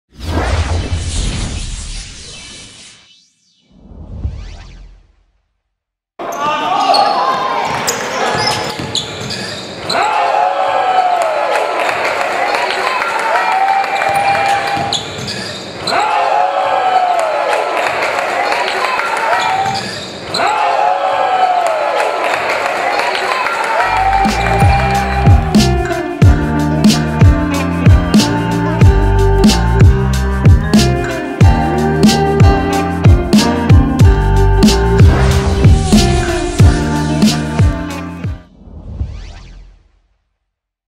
Josh Steel Serves Up a FACIAL & Then Taunts Defender After! Barking Abbey vs Lewisham
Check out Barking Abbey's 1997 born guard, Josh Steel, getting out on the break and throwing it down all over the trailing 6'9" big man who thought he had him!

This took place at the BasketballvsCancer Under-18 Pre-Season Tournament in Solent in the game between Barking Abbey and Lewisham Thunder.

Steel is widely considered the best '97 born prospect in the country and represented the England U16s this summer.